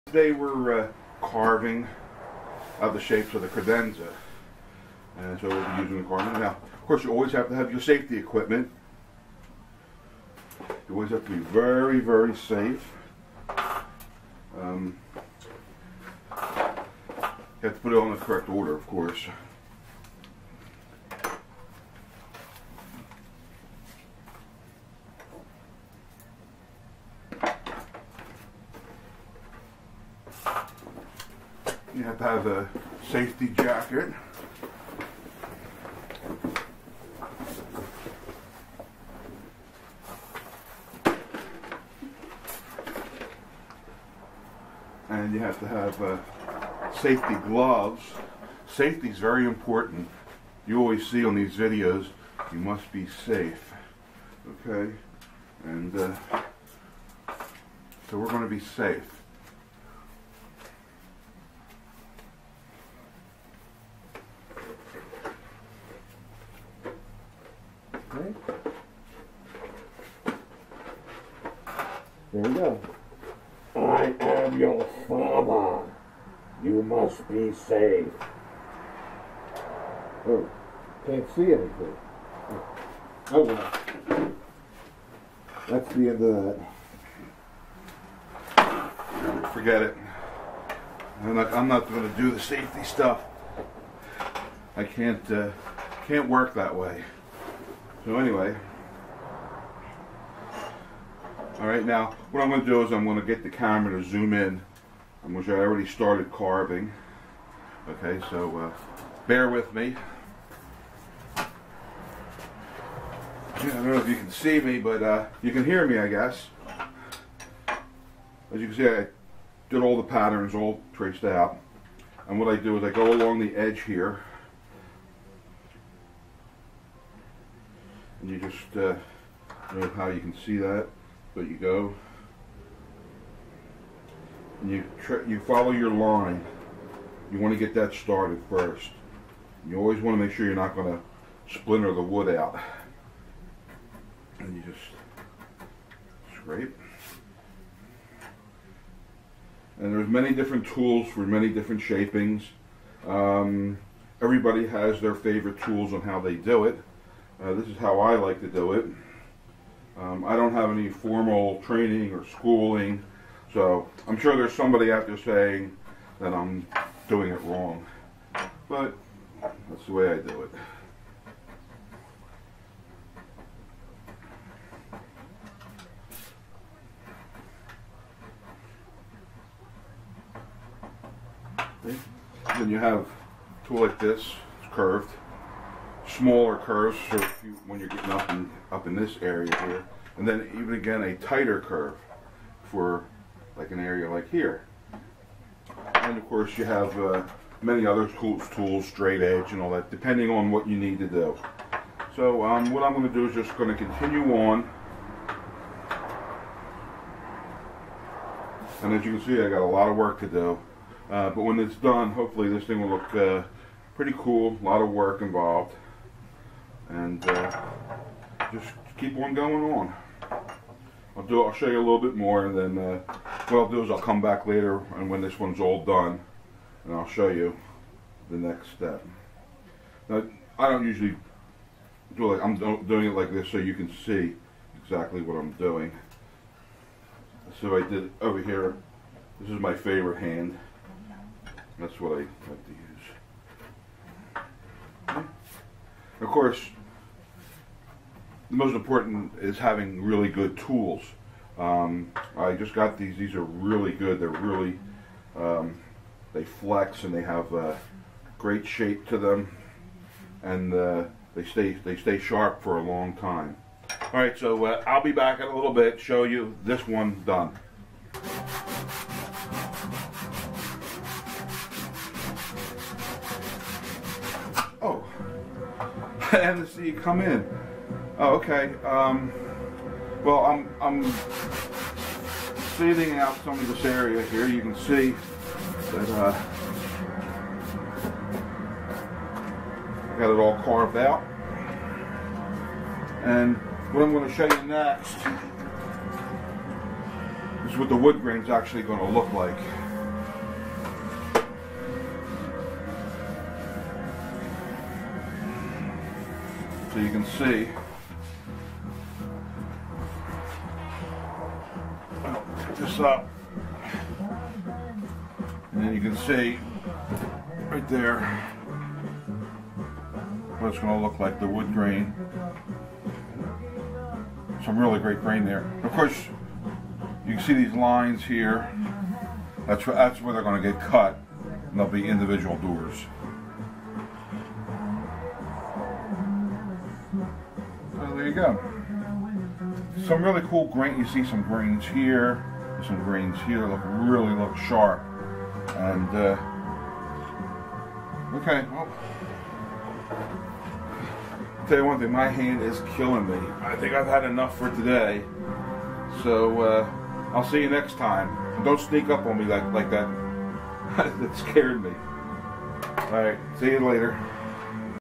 Sculpture Credenza hand wood carving by Garry Zayon
New credenza under construction. I am illustrating how I carve the shapes in to the credenzas doors. Designer artist Garry Zayon Philadelphia Pa